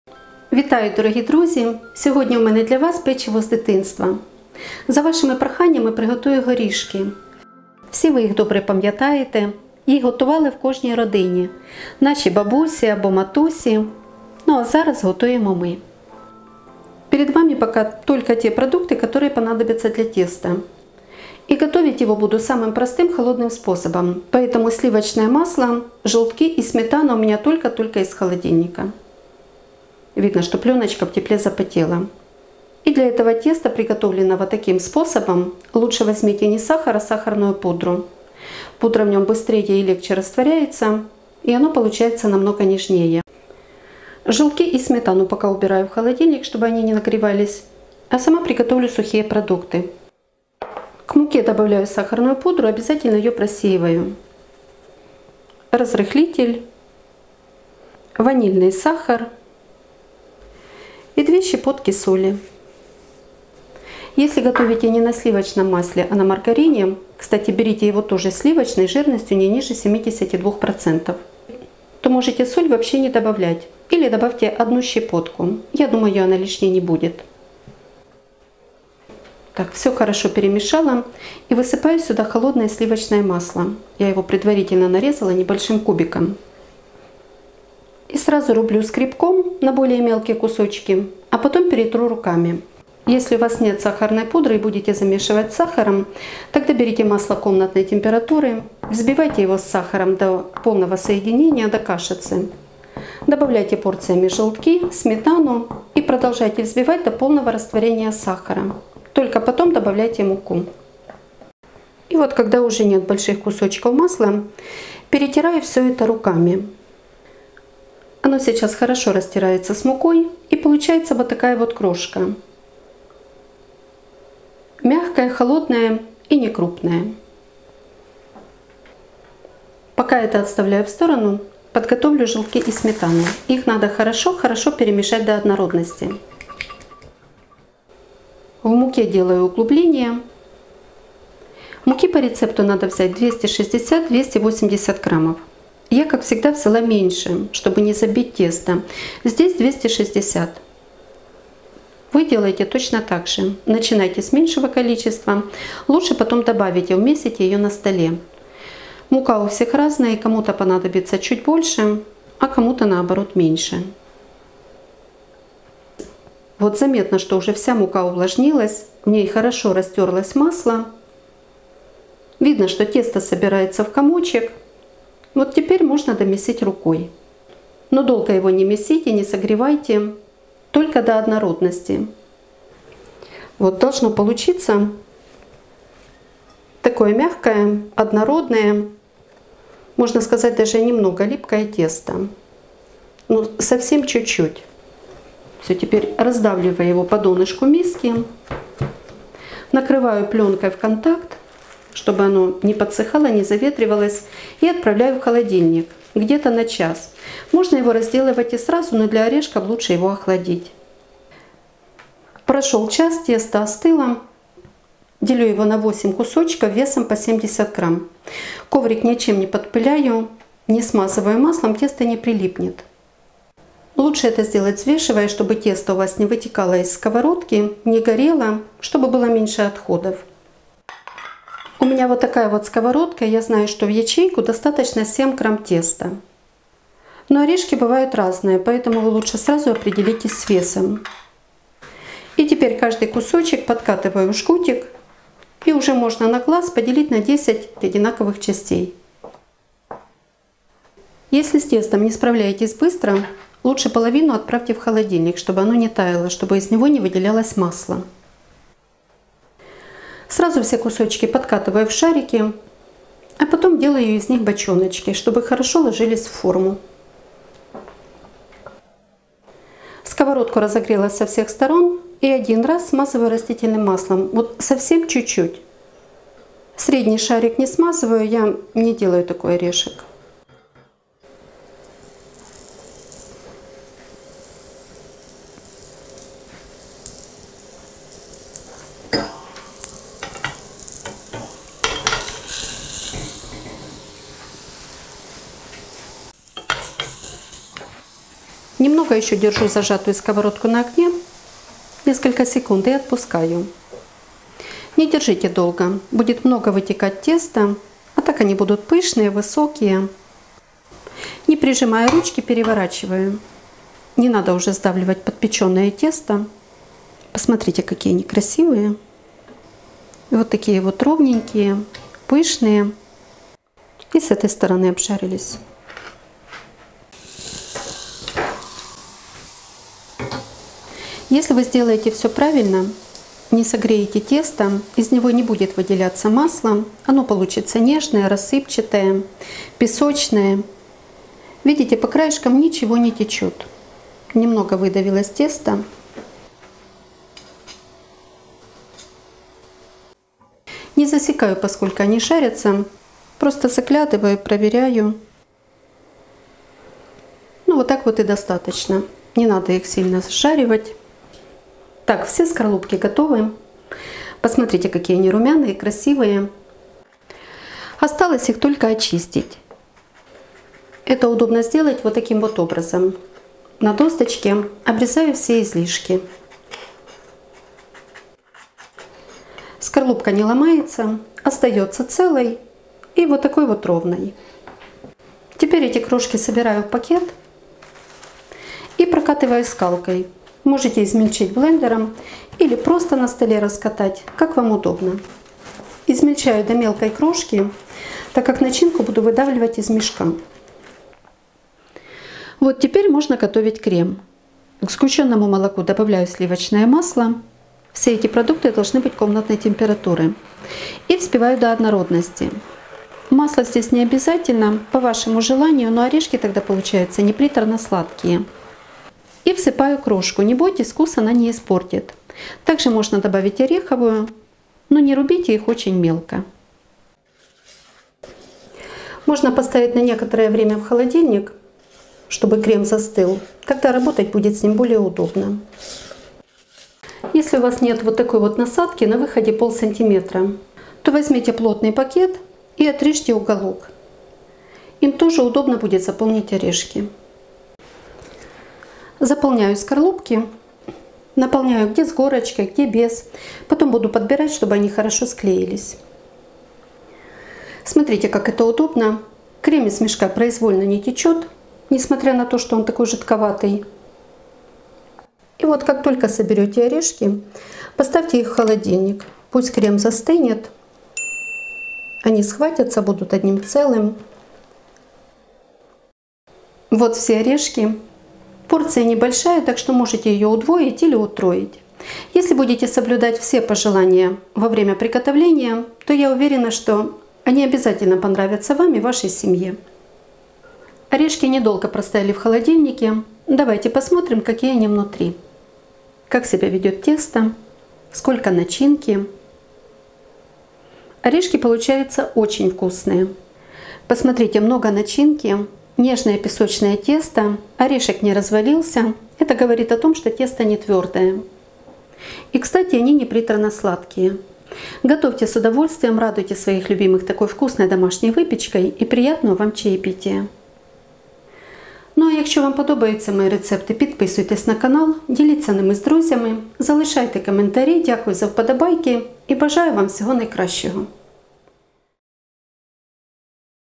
ОРЕШКИ СО СГУЩЁНКОЙ, десерт на все времена /Nuts with condensed milk.
ДОРОГИЕ ДРУЗЬЯ, просмотр рекламы на моих видео это Ваша благодарность за мои труды и помощь в развитии канала. Спасибо за Вашу поддержку.
                                Рецепт:
2 желтка,
140 гр сливочного масла,
100 гр сахарной пудры,
40 гр сметаны,
1 ч. ложка разрыхлителя (без горки),
1 ч. ложка ванильного сахара,
2 щепотки соли,
260-280 гр муки.
                               Рецепт:
300 гр сгущённого молока,
70-80 гр сливочного масла.

                               Recipe:
2 yolks,
140 gr butter,
100 gr of powdered sugar,
40 gr sour cream,
1 teaspoon baking powder (no slide)
1 teaspoon vanilla sugar
2 pinches of salt
260-280 gr flour.
                                Recipe:
300 gr condensed milk,
70-80 gr butter.